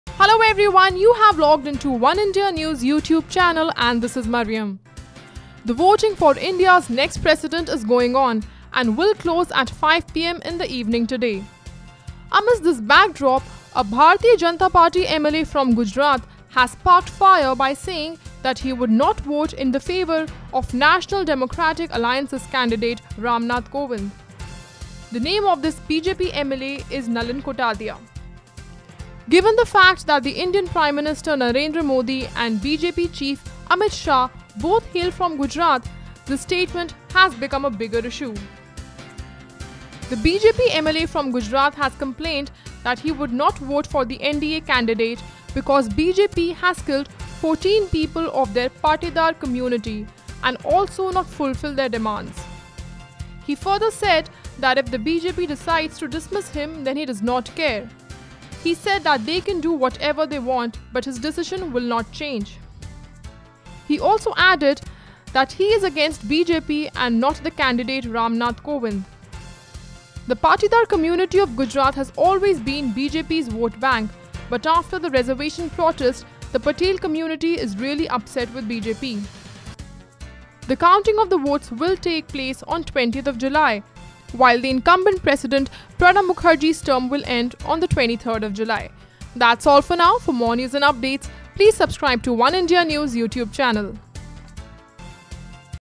Presidential elections: BJP MLA Nalin Koutadia will not vote for Kovind | Oneindia News
The voting for India's next President is going on and will close at 5 pm in the evening today. Amidst this backdrop, A Bhartiya Janata Party MLA from Gujarat, Nalin Koutadiya has sparked  fire by saying that he would vote against the National Democratic Alliance's  candidate Ram Nath Kovind.